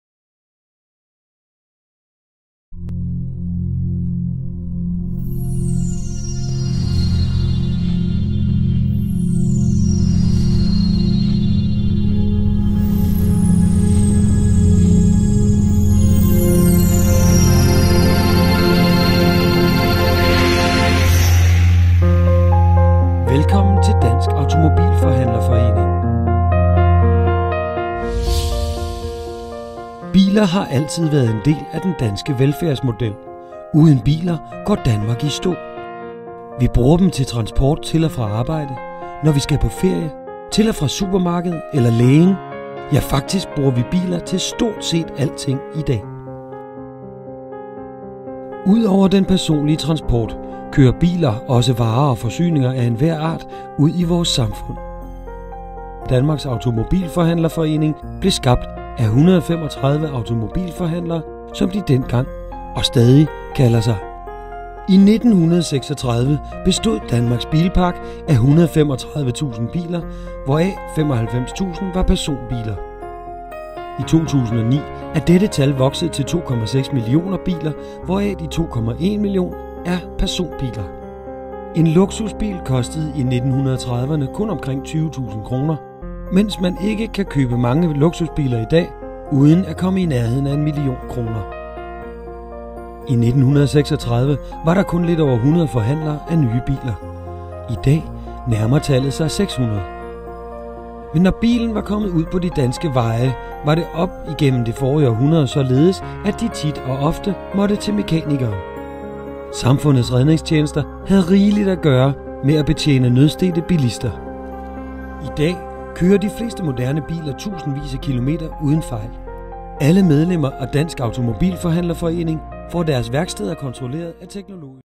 DAF kort.mp4
Storytelling film om Dansk Automobilforhandler Forening fra starten indtil i dag.
Filmen bliver brugt på messer, på web samt til at hverve nye medlemmer.